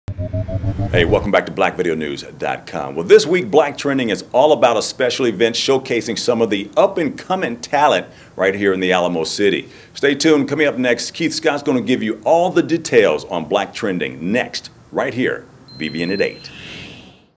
Black Video News Aug 21st 2015 BT
John White gives the scoop on what's coming up on Black Trending with Keith Scott.